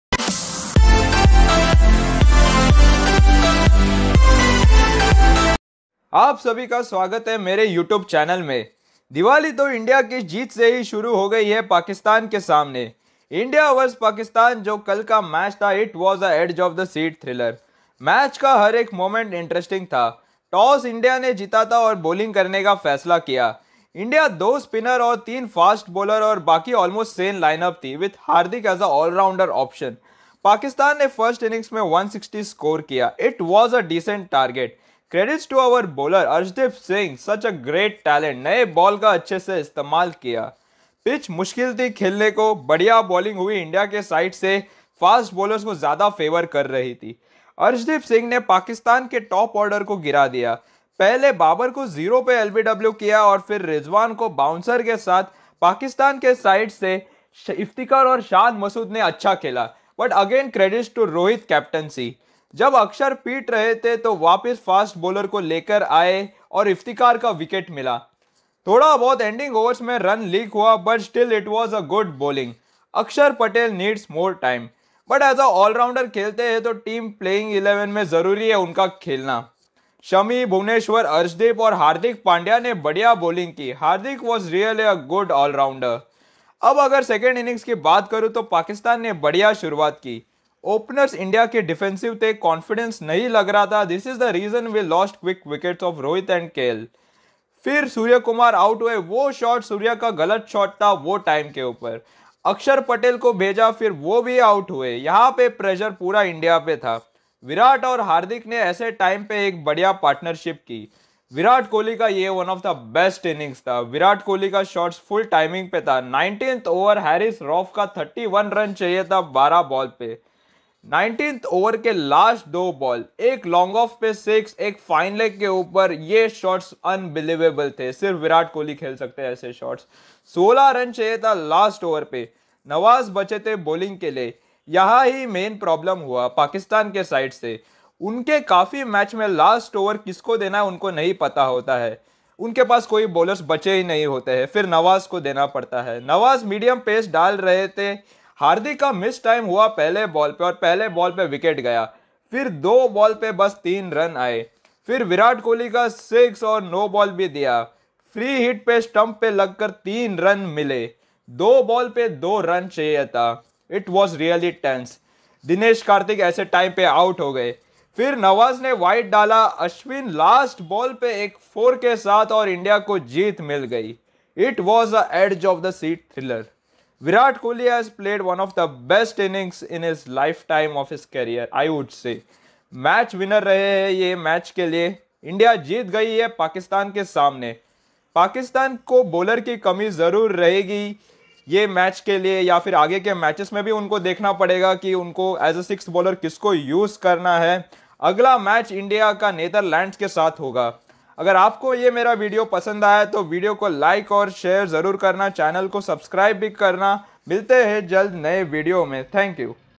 India vs Pakistan t20 World Cup 2022 Highlights | Virat Kohli | Full Review
In this video I discuss about India vs Pakistan t20 World Cup 2022 match, I discuss match points, positive & negative takes from the match.